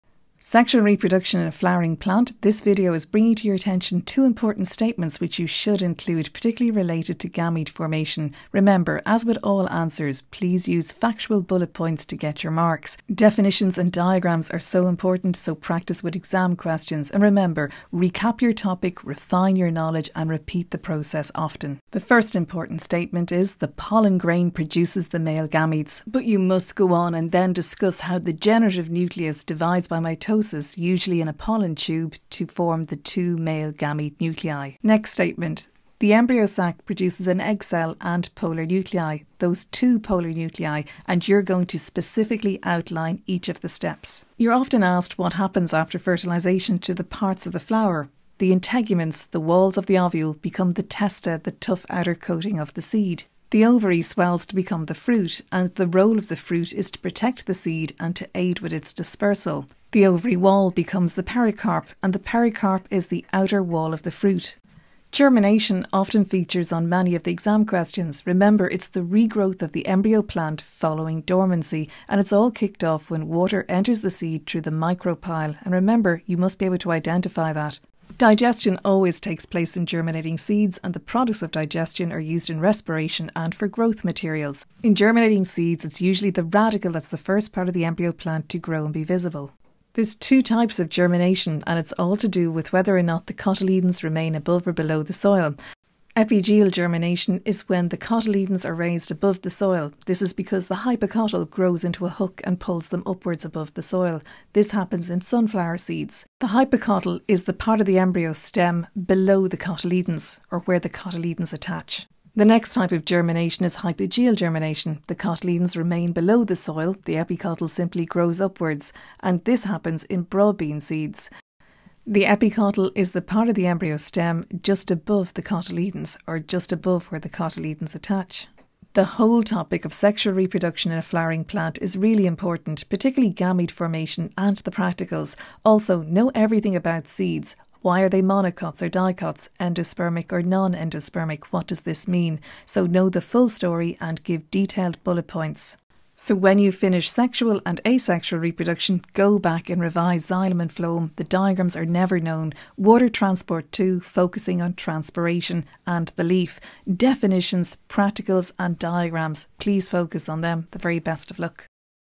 Sexual Reproduction in Flowering Plant-leaving Cert Biology 2025-Revision Prompts
This @BiologyBugbears video is a revision prompt to help focus your revision on key aspects of plant reproduction. Sexual reproduction in flowering plant is essential and should not be skipped-it can yield some easy straight forward questions. Use the numbered videos in the sexual reproduction playlist to learn the content and check in with this video if you can talk around these prompts. These prompts were inspired from the details in the "Guidelines for Teachers book"- Always know every detail and explain fully your answers.